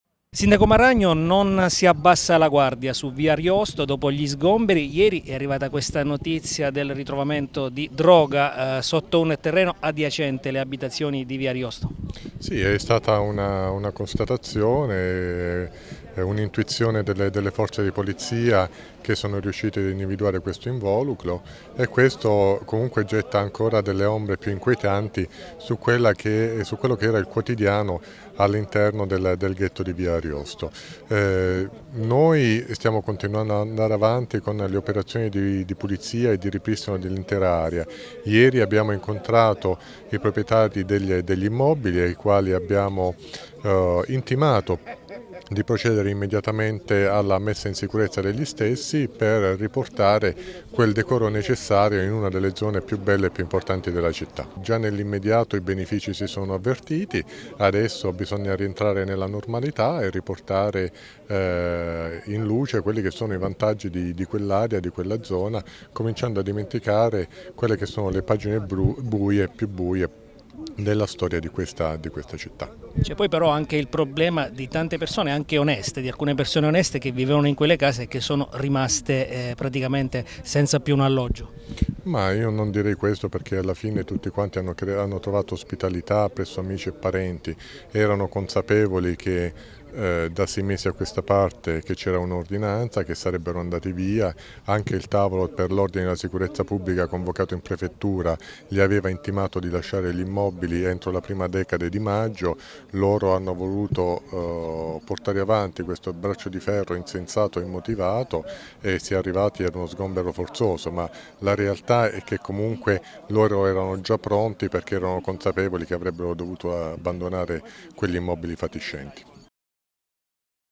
A Montesilvano su via Ariosto non si abbassa la guardia
Dopo gli sgomberi della settimana scorsa in via Ariosto a Montesilvano non si abbassa la guardia. Ieri il ritrovamento di 3,4 kg di marijuana. Il Comune al lavoro per la rivalorizzazione